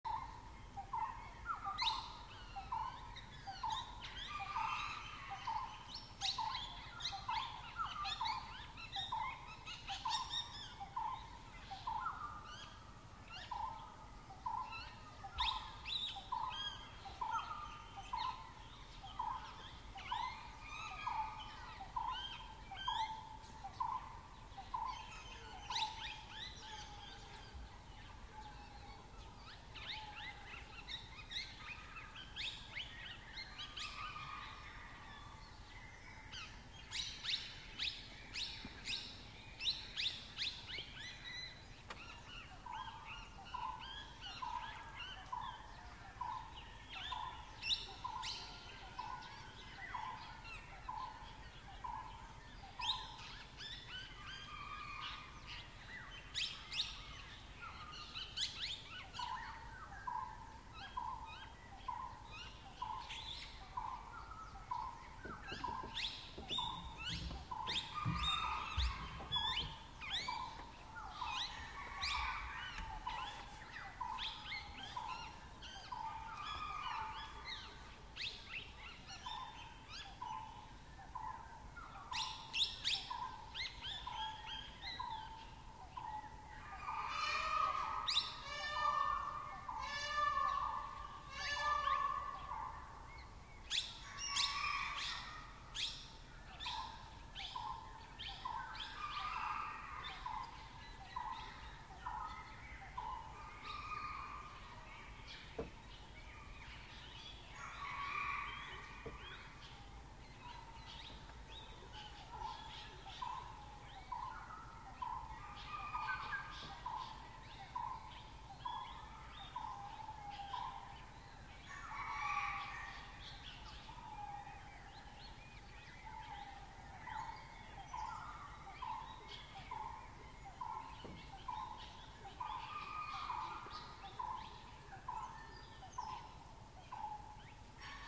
Family of Dholes in Pench Tiger Reserve
I filmed this family of Dholes or Indian Wild Dogs in Pench Tiger Reserve. The pups were a real joy to watch!